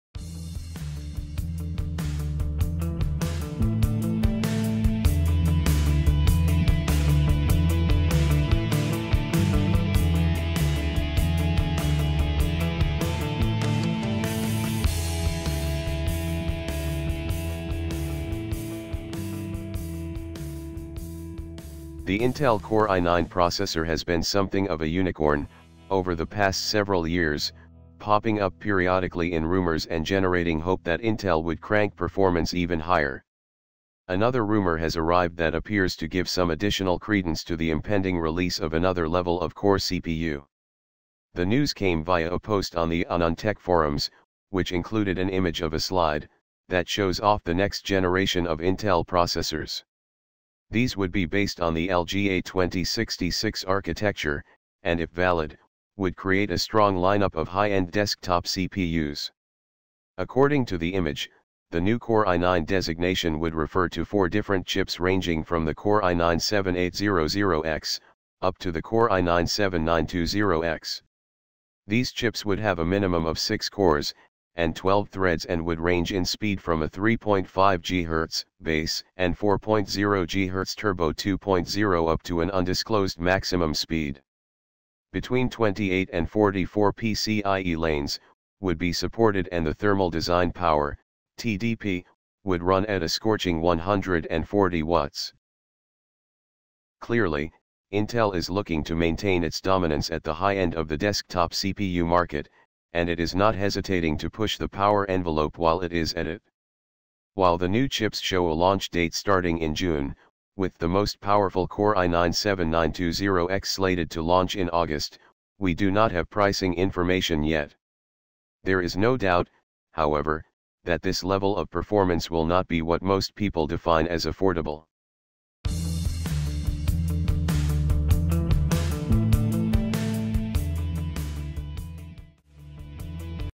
Intel Core i9 Processor
A recent leak of an Intel presentation reveals four new Core i9 chips in the running as Intel's new high-end lineup processors.

The Intel Core i9 processor has been something of a unicorn over the past several years, popping up periodically in rumors and generating hope that Intel would crank performance even higher. Another rumor has arrived that appears to give some additional credence to the impending release of another level of Core CPU.

The news came via a post on the forums, which included an image of a slide that shows off the next generation of Intel processors. These would be based on the LGA-2066 architecture, and if valid, would create a strong lineup of high-end desktop CPUs, Techspot reports.

According to the image, the new Core i9 designation would refer to four different chips ranging from the Core i9-7800X up to the Core i9-7920X. These chips would have a minimum of six cores and 12 threads and would range in speed from a 3.5GHz base and 4.0GHz Turbo 2.0 up to an undisclosed maximum speed. Between 28 and 44 PCIe lanes would be supported and the thermal design power (TDP) would run at a scorching 140 watts.

Upcoming high-end Skylake-X and Kaby Lake-X processors

Clearly, Intel is looking to maintain its dominance at the high-end of the desktop CPU market and it is not hesitating to push the power envelope while it is at it. While the new chips show a launch date starting in June, with the most powerful Core i9- 7920X slated to launch in August, we do not have pricing information yet. There is no doubt, however, that this level of performance will not be what most people define as “affordable.”

Intel Processors & Turbo 4.50
Intel Cube Core i9
Kaby Lake-X
Skylake-X
12-core Core i9
AMD's 16-core flagship
Threadripper
AMD Processor